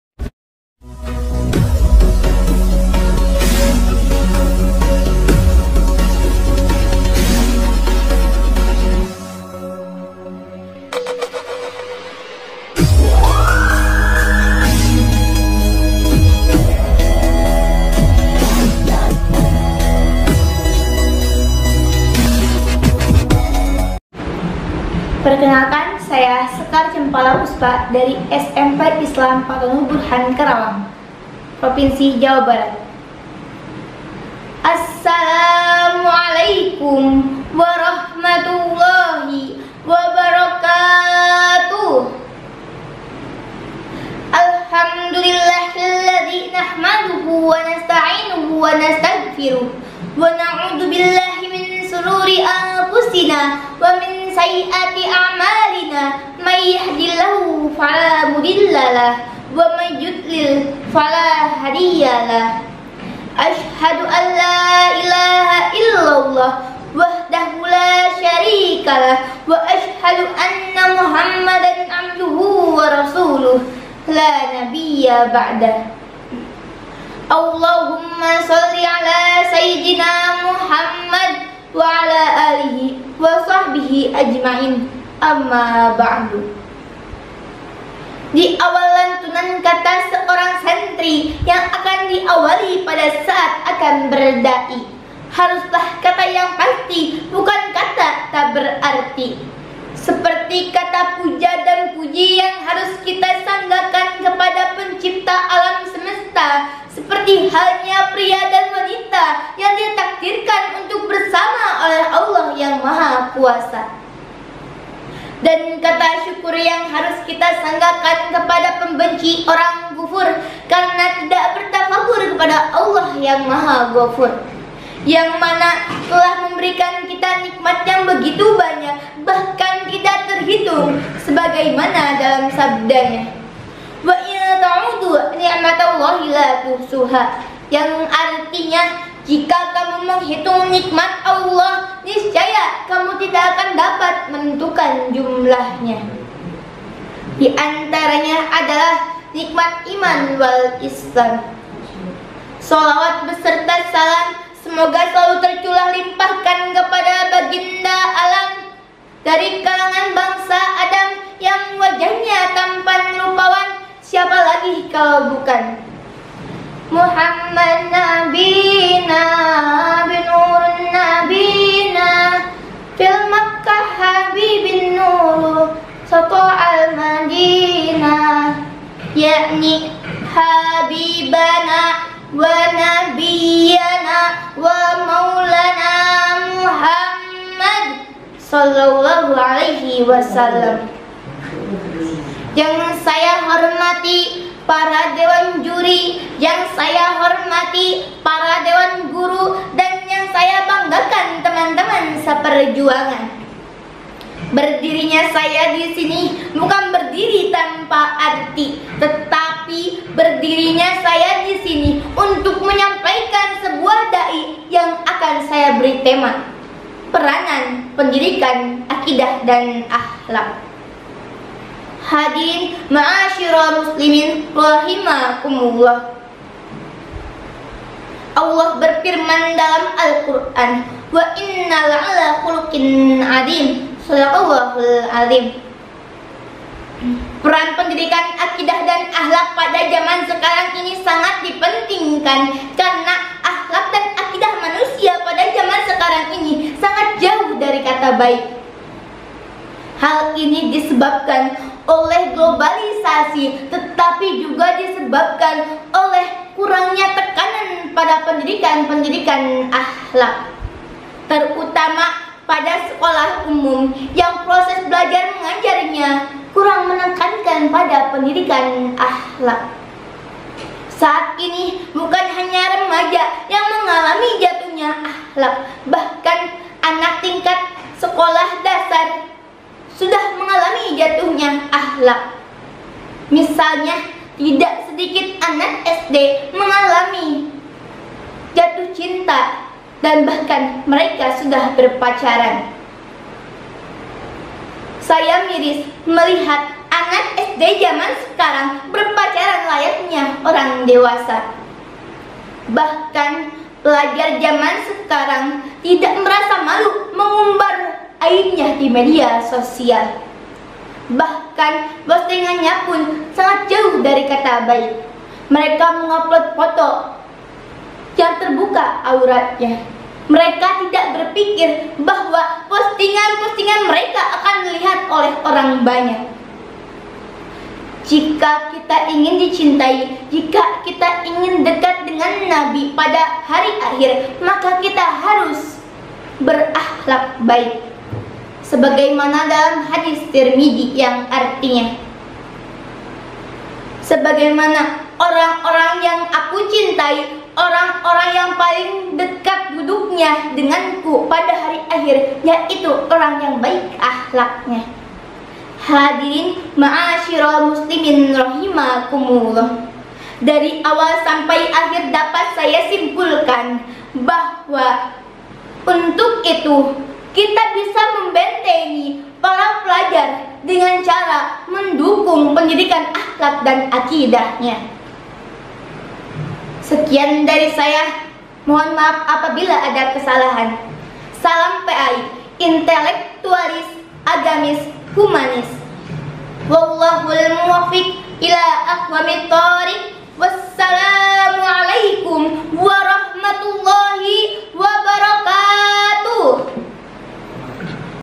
SEKAR CEMPALA PUSPA || Haflah Miladiyah PAI XXVIII || Lomba Da'i Da'iah Tingkat Nasional 2021
Assalaamu'alaikum Warahmatullahi Wabarokatuh

Halo sobat PAI yang dirahmati Allah.
Akhirnya kita bertemu lagi, dan kiranya tak lupa mengucap "Alhamdulillahilladzii bini'matihi tatimmus sholihaat" atas segala kebaikan yang semoga selalu Allah limpahkan kepada kita.

Berikut merupakan Video Lomba Da'i-Da'iah tingkat Nasional dengan tema “Pendidikan Aqidah Akhlaq dalam Pembentukan Karakter Generasi Muda” sebagai penyemarak Haflah Miladiyah Pendidikan Agama Islam UINSA ke - 28.
Salah satu peserta Lomba Da'i-Da'iah tingkat Nasional Haflah PAI 2021 ini ialah :

Nama                 : SEKAR CEMPALA PUSPA
Asal Instansi     : SMPI FATHONUL BURHAN KARAWANG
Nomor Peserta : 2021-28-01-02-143

Jaga Iman, Jaga Imun, Tetap Aman.
Semoga Allah selalu melindungi dan merahmati kita sekeluarga.
Aamiin yaa Mujiibassaailiin 🤲🏻

Regards. Panitia Haflah Miladiyah PAI Ke-XXVIII UINSA.

🌸 Salam PAI 🌸
Intelektualis. Agamis. Humanis.